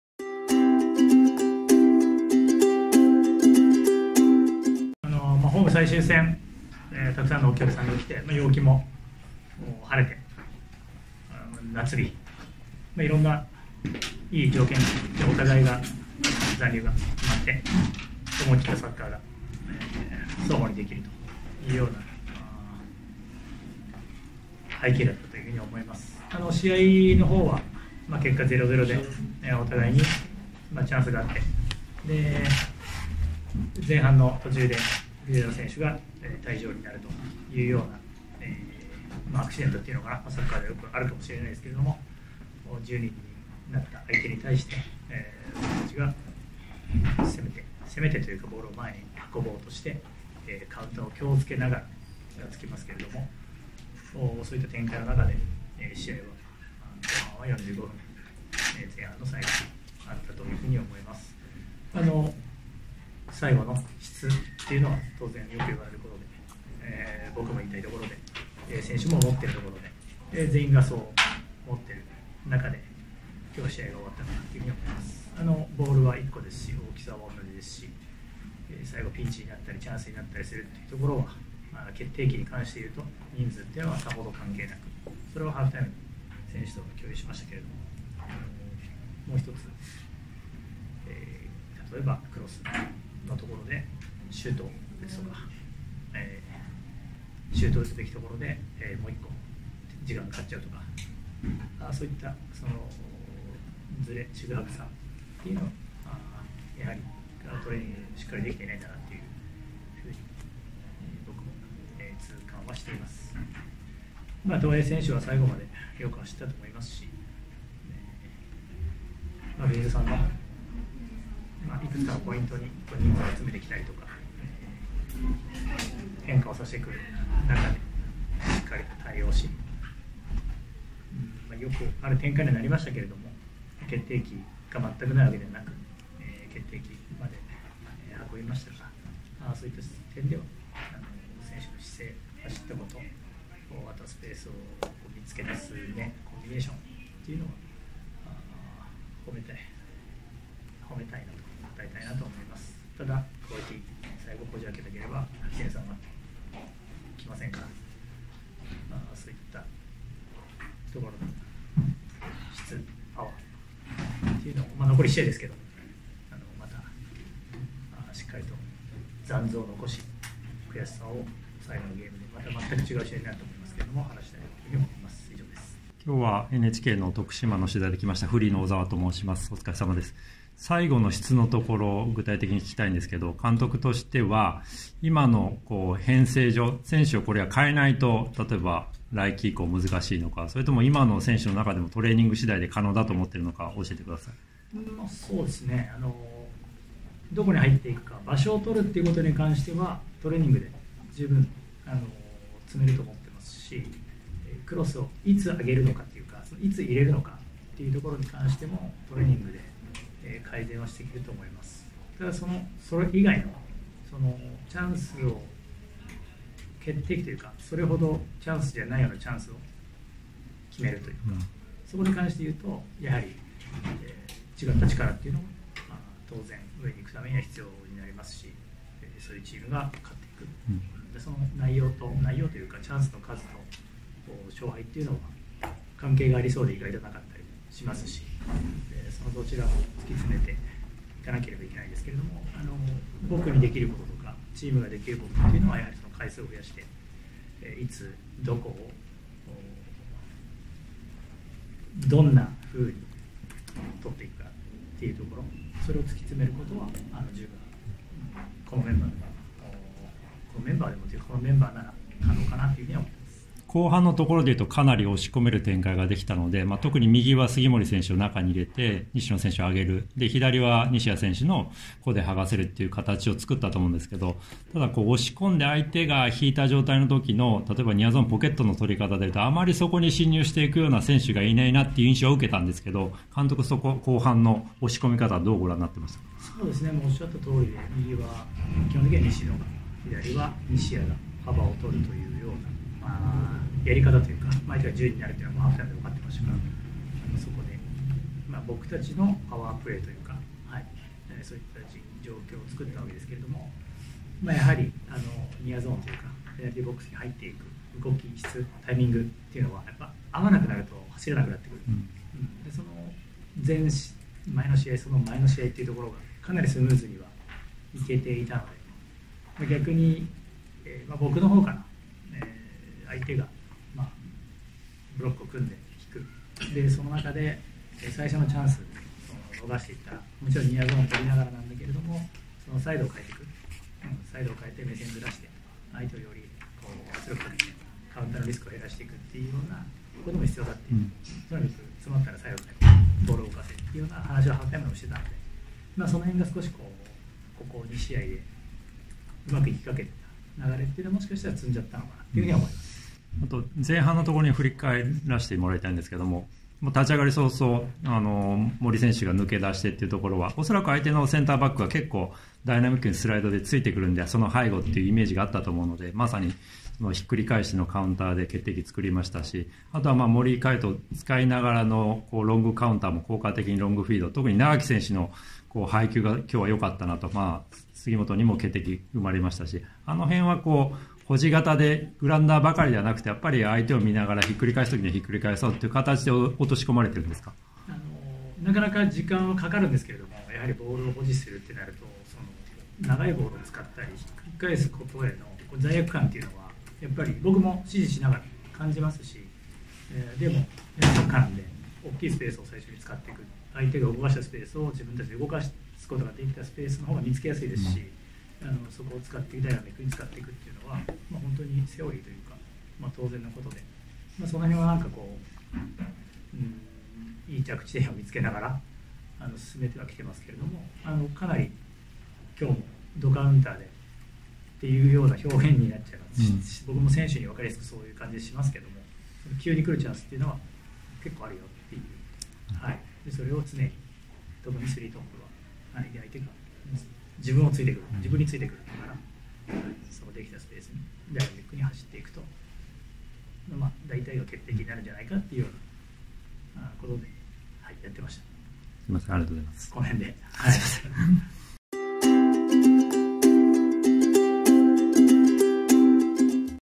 10人相手に無得点の徳島。会見で吉田達磨監督に「最後の質」について3つ質問してきました。｜J2 第41節 徳島 vs 藤枝
取材日：23年11月4日

J2 第41節 徳島（0-0）藤枝 試合後監督会見
徳島ヴォルティス 吉田達磨監督

2:56  質問1
4:23  質問2
5:53  質問3